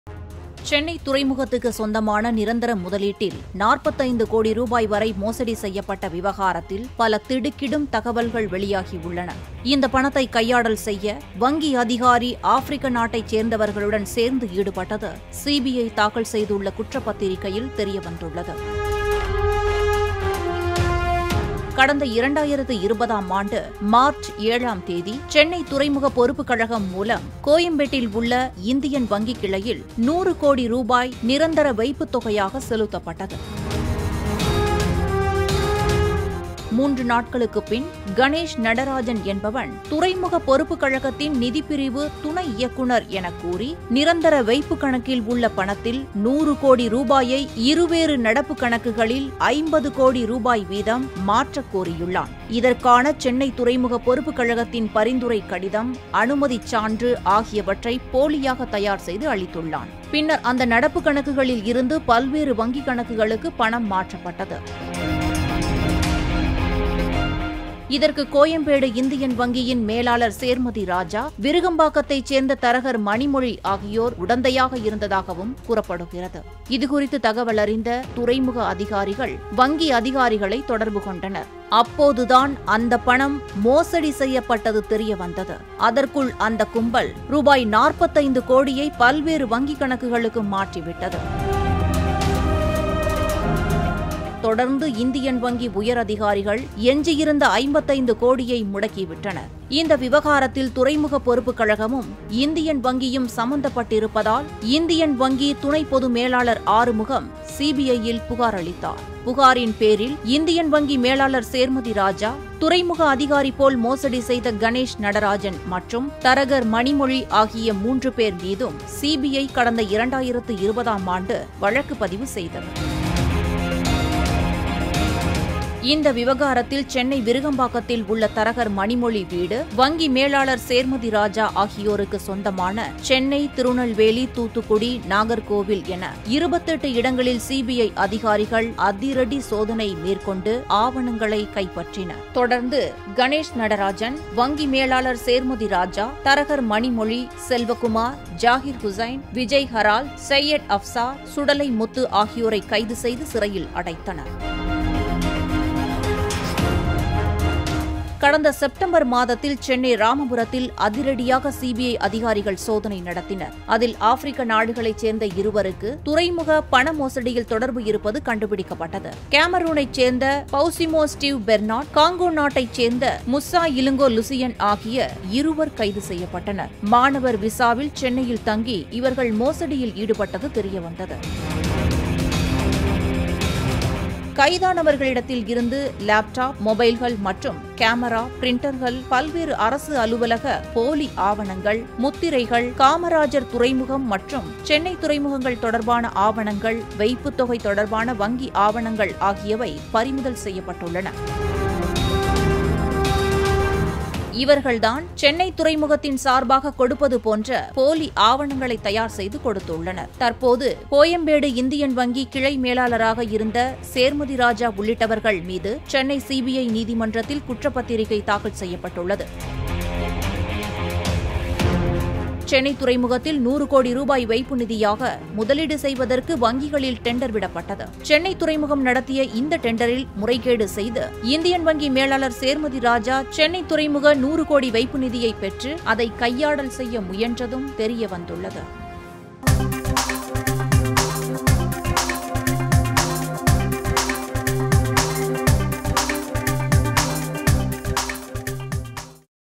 வங்கிக்குள் ஒரு வில்லன்.. கைகோத்த இரு ஆப்ரிக்க நாட்டவர்கள்.. வெளியான அதிர்ச்சித்தகவல்கள்!
சென்னை துறைமுகத்துக்கு சொந்தமான 45 கோடி மோசடி வங்கிக்குள் ஒரு வில்லன்... கைகோத்த இரு ஆப்ரிக்க நாட்டவர்கள்... வெளியான அதிர்ச்சித்தகவல்கள்!